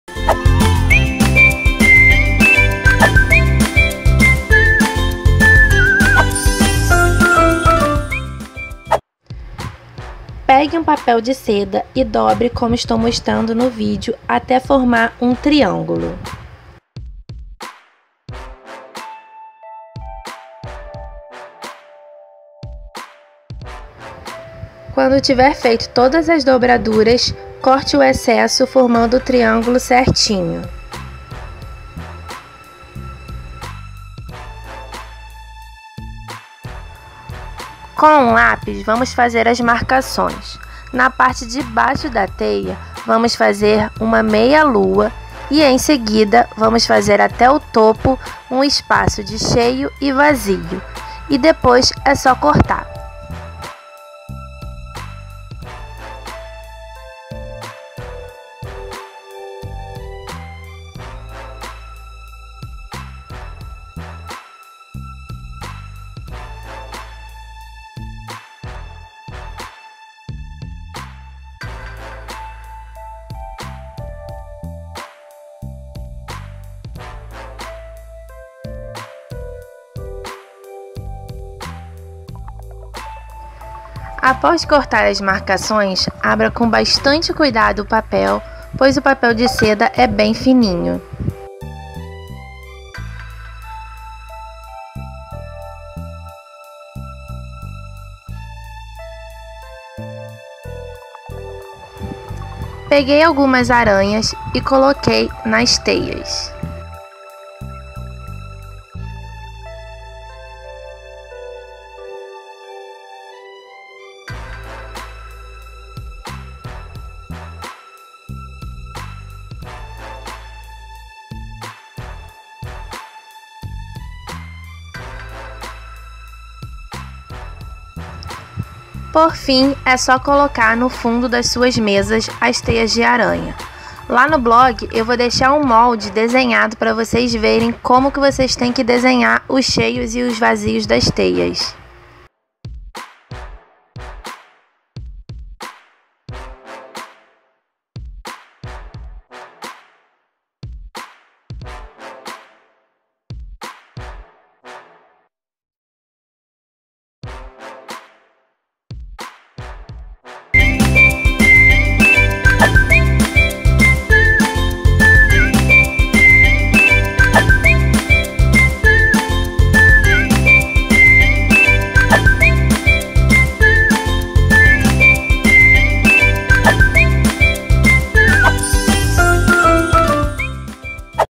COMO FAZER TEIA DE ARANHA DE PAPEL PARA DECORAR - FAÇA SUA FESTA | POR CAROL GOMES
Nesse vídeo mostro como fazer teia de aranha de papel para decorar festa de Halloween.

♡ENGLISH:

In this video I show how to make paper spider web to decorate Halloween party.

HOW TO MAKE PAPER SPIDER WEB FOR DECORATION - MAKE YOUR PARTY | BY CAROL GOMES

♡Español:

En este video muestro cómo hacer tela de araña de papel para decorar la fiesta de Halloween.

CÓMO HACER PAPEL SPIDER WEB PARA DECORACIÓN - HAGA SU FIESTA | POR CAROL GOMES